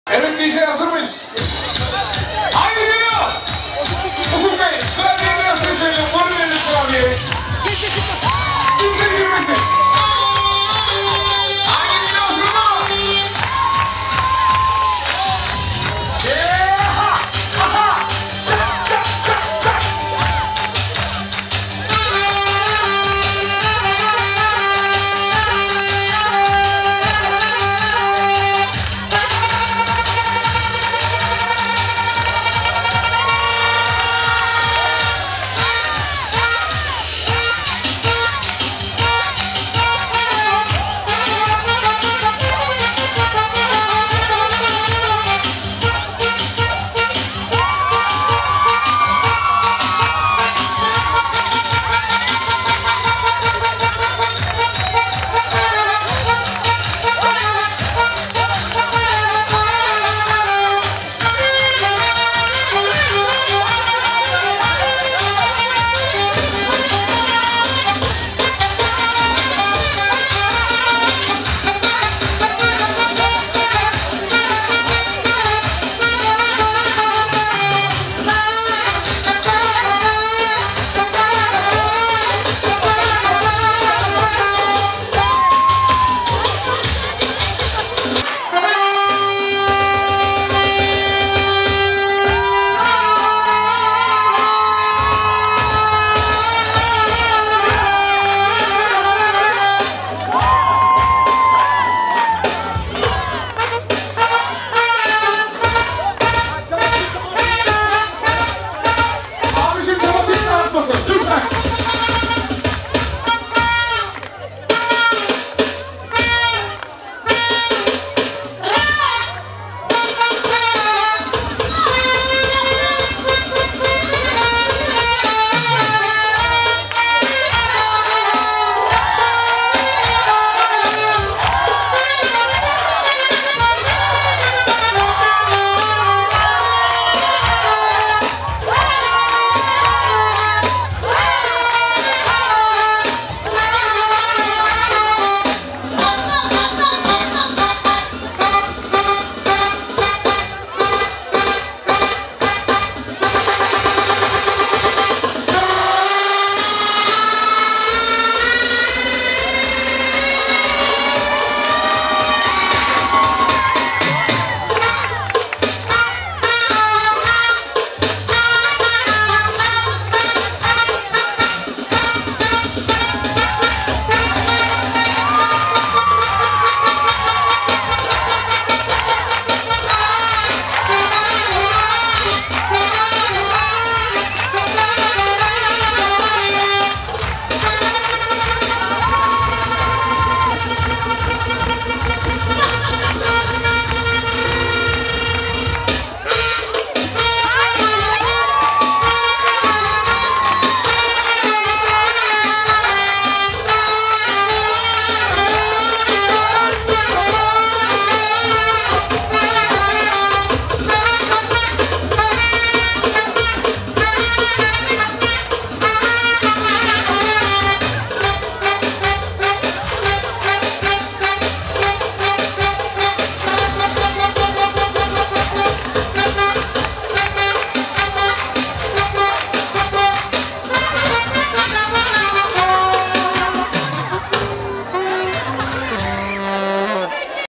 Roman Düğünleri Söz Konusu Roman Kızı Nuriye Ateş Olunca Daha Önce Görülmemiş Çat Çat Show
Yeni Videolarımdan Anında Haberdar Olmanız İçin